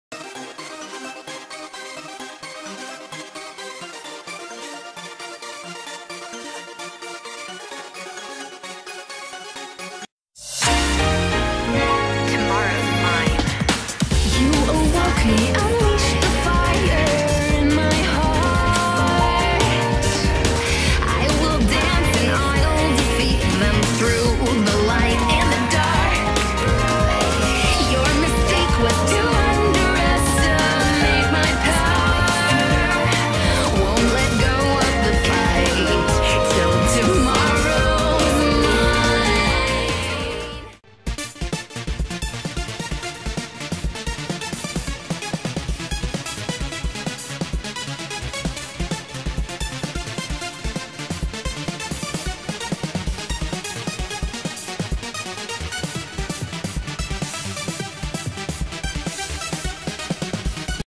JatoStyle Crossover mini animation 4-18-21
more like a short animation test but I just wanted to try something small,
I am learning to not over animate and add too much details.